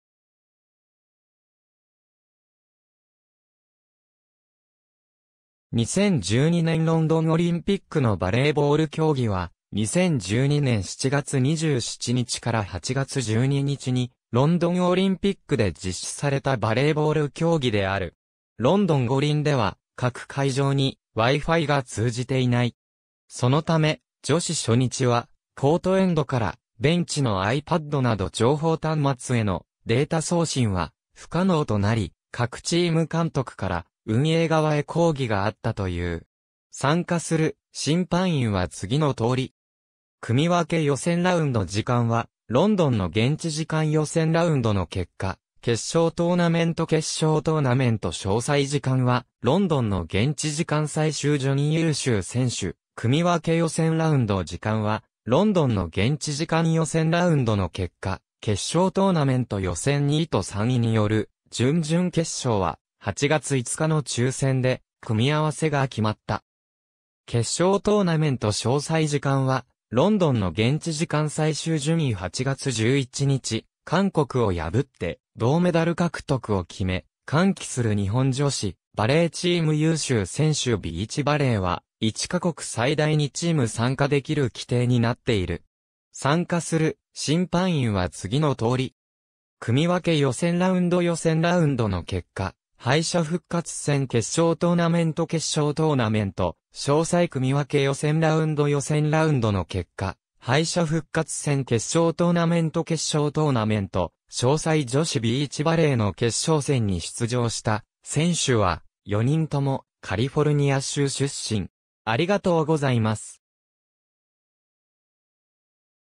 2012年ロンドンオリンピックのバレーボール競技
ケンジントン・アンド・チェルシー区
シティ・オブ・ウェストミンスター
2012年ロンドンオリンピックのバレーボール競技（2012ねんロンドンオリンピックのバレーボールきょうぎ）は、2012年7月27日 - 8月12日に、ロンドンオリンピックで実施されたバレーボール競技である。
ロンドン五輪では、各会場にWi-Fiが通じていない（「時計の計測に影響する」などの理由から）。
そのため、女子初日（28日）はコートエンドからベンチのiPadなど情報端末への、データ送信は不可能となり、各チーム監督から運営側へ抗議があったという。
参加する審判員は次の通り。
組み分け 予選ラウンド ※ 時間はロンドンの現地時間 予選ラウンドの結果 決勝トーナメント 決勝トーナメント詳細 ※ 時間はロンドンの現地時間 最終順位 優秀選手 組み分け 予選ラウンド ※ 時間はロンドンの現地時間 予選ラウンドの結果 決勝トーナメント 予選2位と3位による準々決勝は、8月5日の抽選で組合せが決まった。
決勝トーナメント詳細 ※ 時間はロンドンの現地時間 最終順位 8月11日、韓国を破って銅メダル獲得を決め、歓喜する日本女子バレーチーム 優秀選手 ビーチバレーは、1カ国最大2チーム参加できる規定になっている。
組み分け 予選ラウンド 予選ラウンドの結果 敗者復活戦 決勝トーナメント 決勝トーナメント詳細 組み分け 予選ラウンド 予選ラウンドの結果 敗者復活戦 決勝トーナメント 決勝トーナメント詳細 ※女子ビーチバレーの決勝戦に出場した選手は、4人ともカリフォルニア州出身。